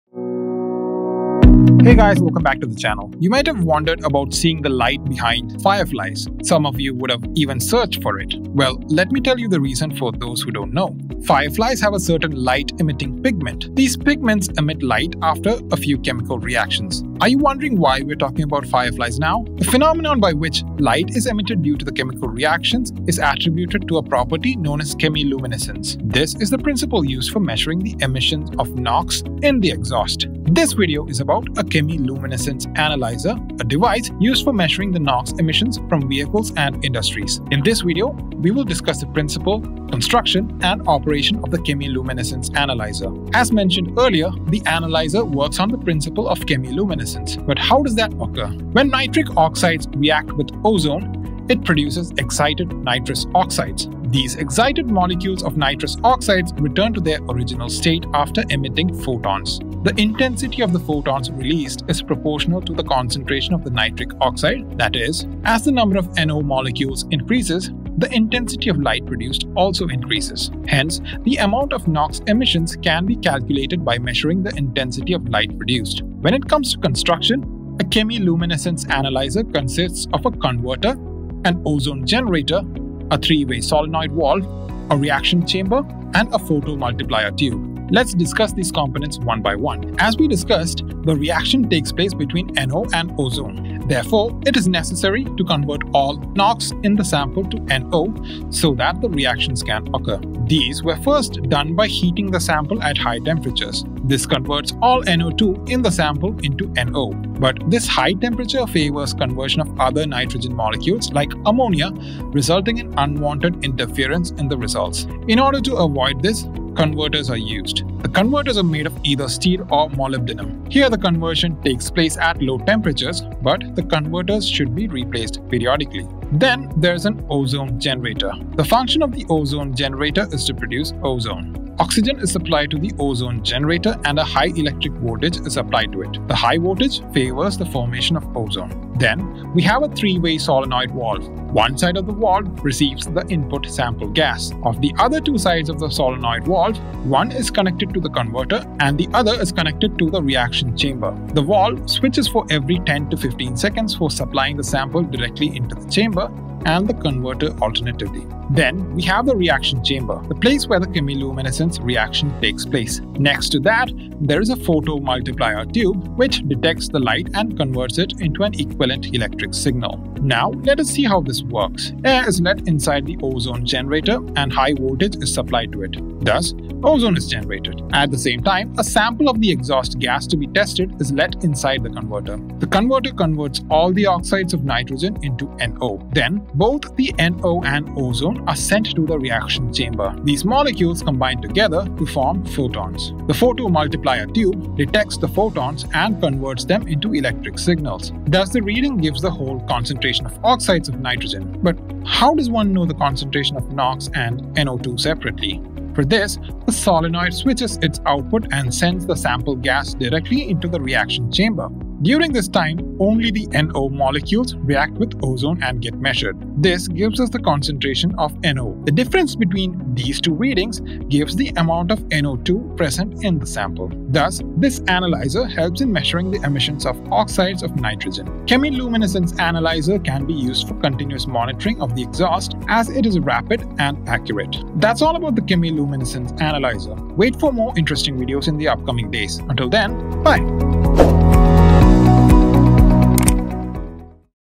Chemiluminescence Analyser | Skill-Lync
You might have seen the light behind fireflies. Fireflies have a certain pigment that emits light after some chemical reactions. This phenomenon is known as Chemiluminescence.

The same phenomenon is used to detect and measure the emissions of NOx from engines and industries and the device used for this is known as Chemiluminescence Analyser.

In this video, we have explained what a chemiluminescence analyser is, its construction and how it works.

When nitric oxides (NO) react with ozone, it produces excited nitrous oxides
(NO2*). These excited molecules of nitrous oxides return to their original state after emitting photons. The intensity of the photons released will be proportional to the concentration of the nitric oxide. Therefore, by measuring the intensity of light produced, the NO emission can be measured.

The analyser consists of an ozone generator which generates ozone and a converter which converts all the molecules of NOx in the sample into NO. When the ozone and nitric oxide react inside the reaction chamber, photons are produced and the produced photons are detected by the photomultiplier tube. The photomultiplier tube converts the light energy into electrical energy and then sends it to the display. This shows the total concentration of NOx.

The analyser also uses a three way solenoid valve which can supply the sample directly into the chamber bypassing the converter. This condition will give us the concentration of NO and the difference between the first two readings gives us the concentration of NO2.

Chemiluminescence Analysers are used for continuous monitoring of the exhaust as it is rapid and accurate.

About Skill-Lync